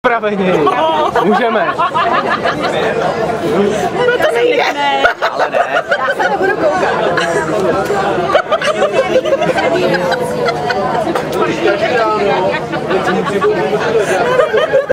Vaping Party Boat - PETRUŠE
Tady jsem asi předpojatý, páč mám slabost pro zrzky... ( klíííd, i pro brunetky a blondýnky...:-) ) Nicmně určitě mi dáte za pravdu že jedna z nejpůvabnějších e-vapic...:-)